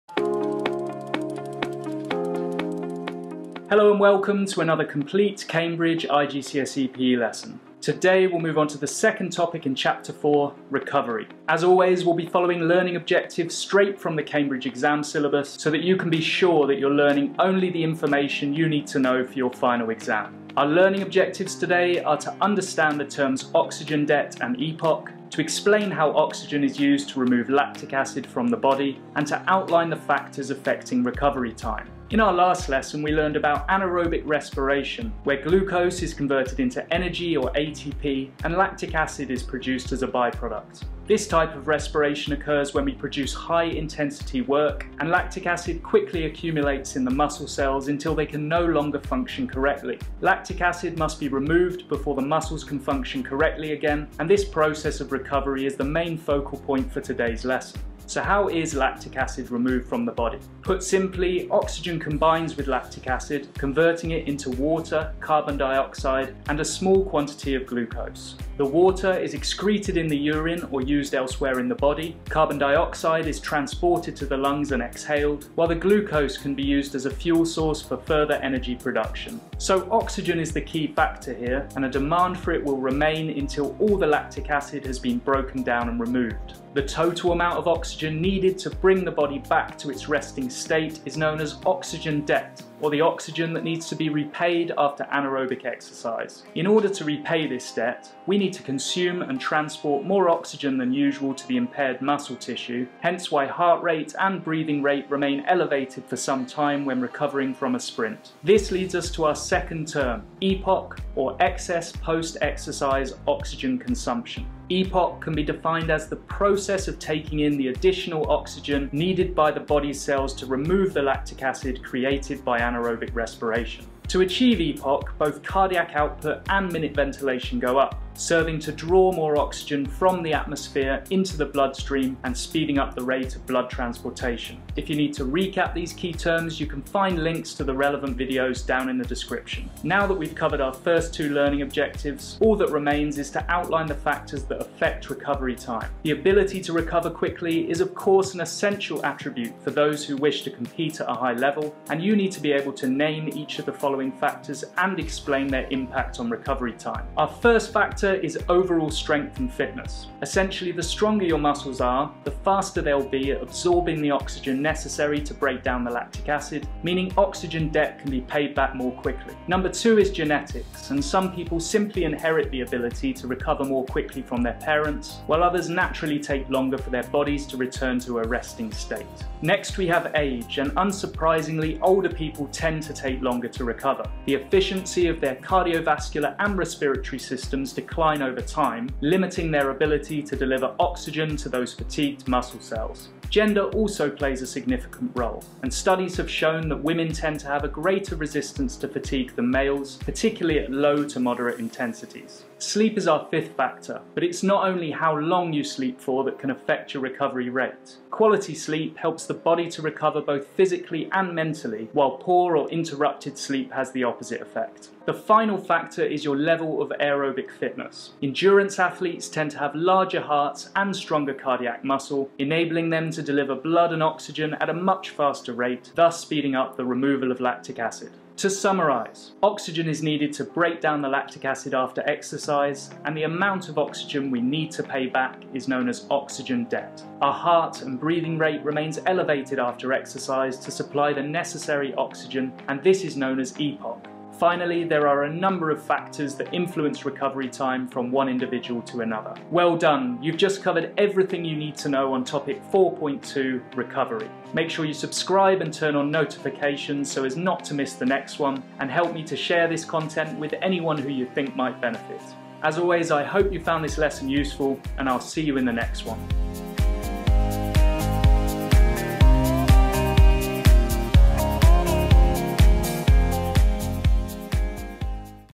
GCSE PE - RECOVERY, OXYGEN DEBT & EPOC - Anatomy and Physiology (Energy & Exercise Effects - 4.2)
Lesson #14 of the Cambridge IGCSE PE syllabus.
Topic: Anatomy and Physiology
Chapter: Energy Supply and the Effects of Exercise on the Body
Lesson 14: Recovery